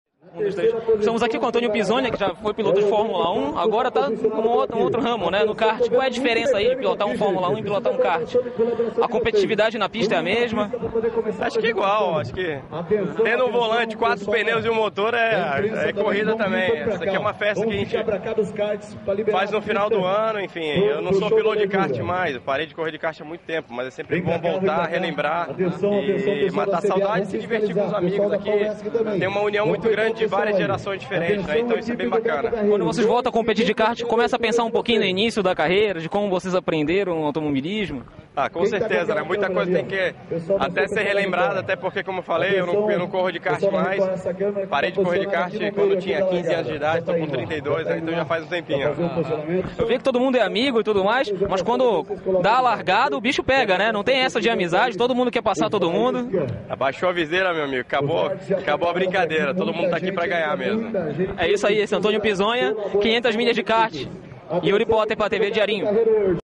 Trio da Spirit Sport desbanca estrelas e vence 500 Milhas de Kart do Beto Carrero World
Foram necessárias exatas 11 horas, 39 minutos e 21 segundos para que o kart de número 13, da equipe Spirit Sport, completasse as 650 voltas e se sagrasse campeão da 16ª edição do desafio

Leia a matéria na edição de hoje, dia 17 de dezembro de 2012, no